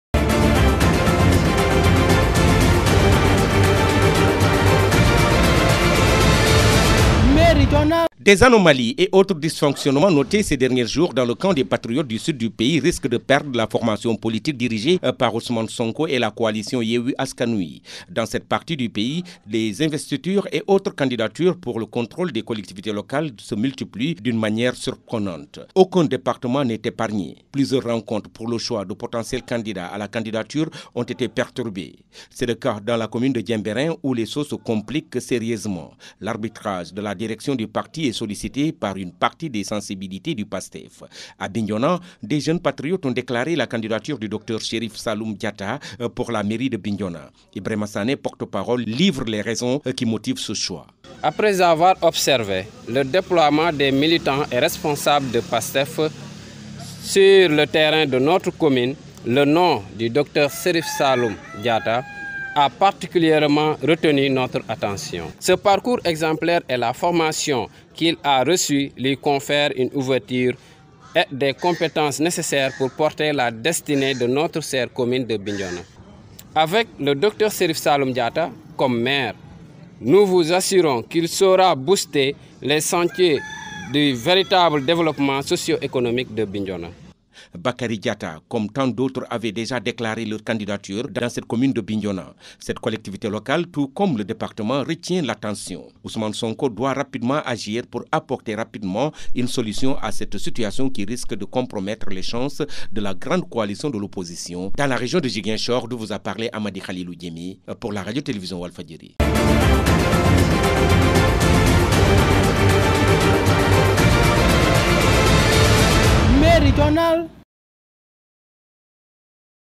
LOCALES 2022, des anomalies et autres dysfonctionnements risquent de perdre le Pastef
Dans la région sud du pays, les investitures et autres candidatures pour le contrôle des collectivités locales de multiplient d’une manière surprenante dans le camp des patriotes engendrant des tiraillements et autres. Aucun département n’est épargné. Plusieurs rencontres pour le choix de potentiels candidats à la candidature ont été perturbées. Des anomalies et dysfonctionnements qui risquent de perdre la formation politique dirigée par Ousmane et la Coalition Yewi Askan Wi.